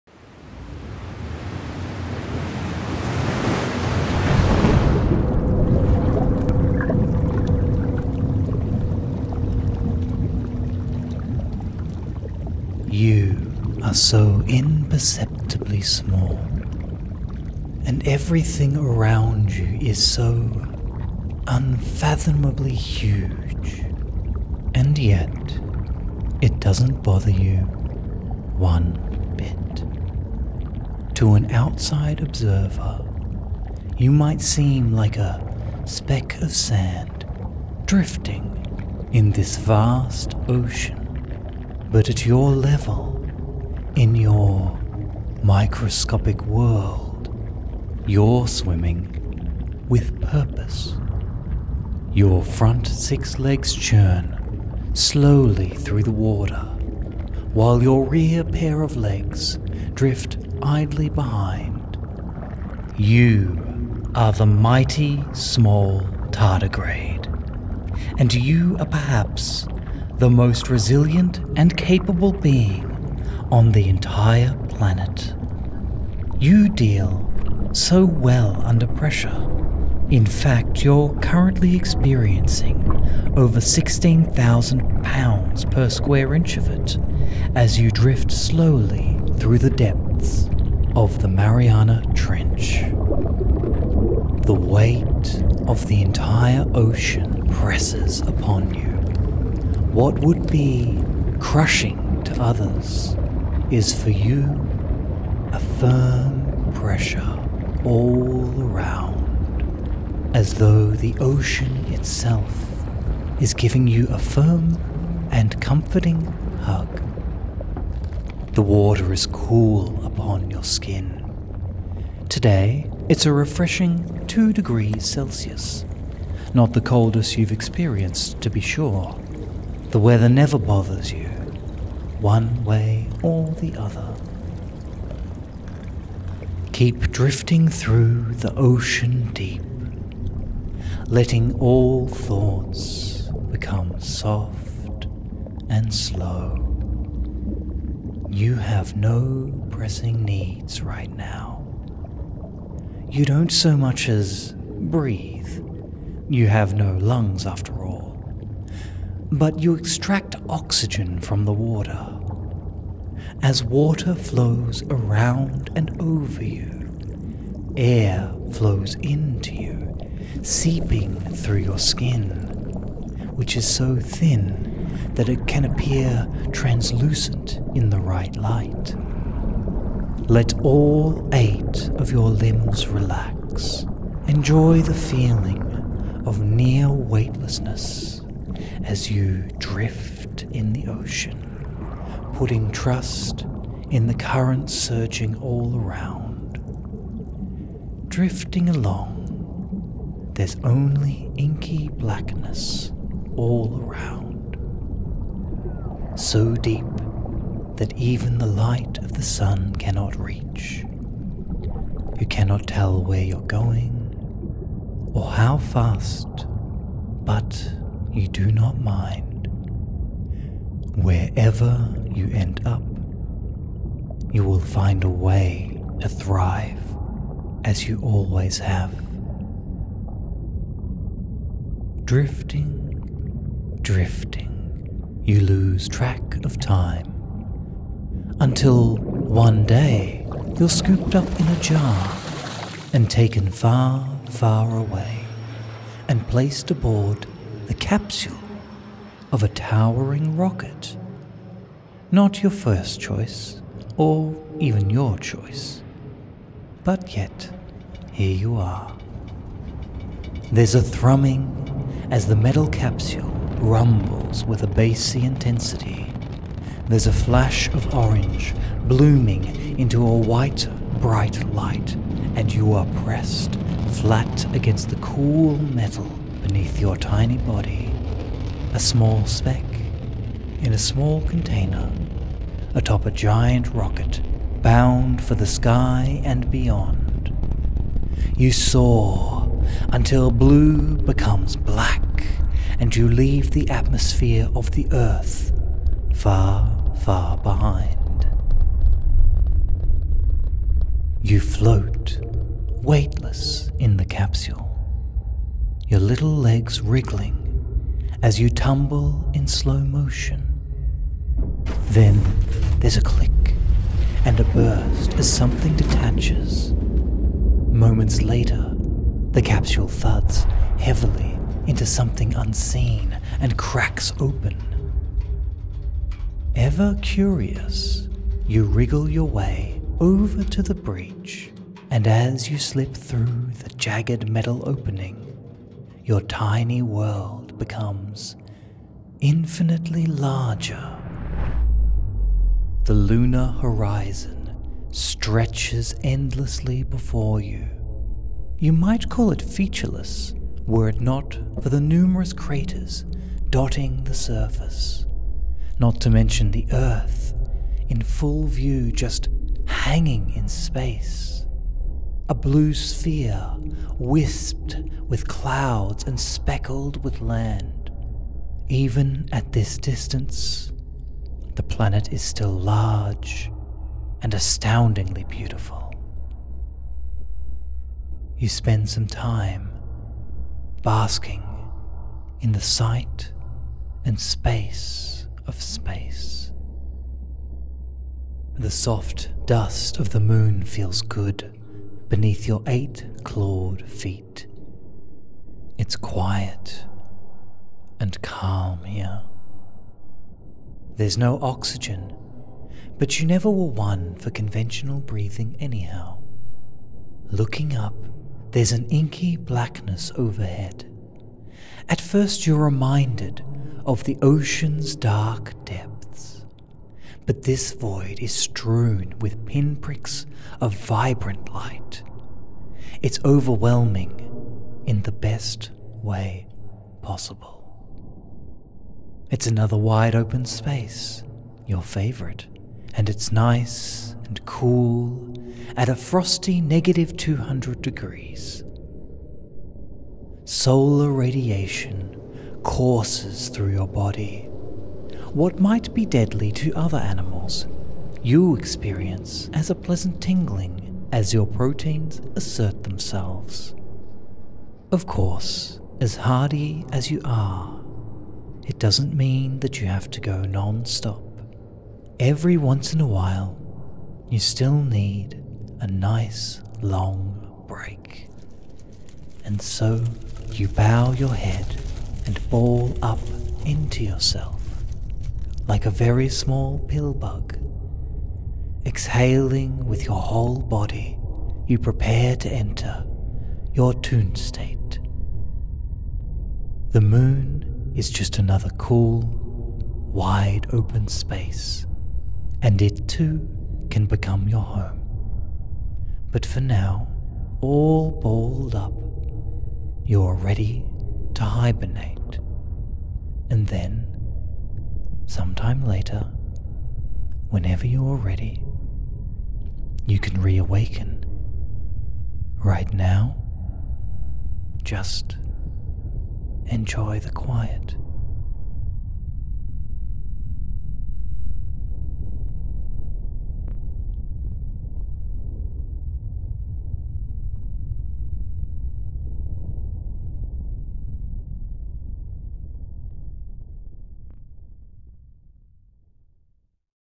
Mighty Small [Tardigrade relaxation audio]
You are a tiny tardigrade, floating in the endless expanse of the ocean.

I've tried to make this audio as scientifically accurate as possible. There's a trend in stories about the tardigrade to report on how much they can survive in experimentation, but I wanted to spend a large amount of time focusing on the places in which tardigrades choose to live in.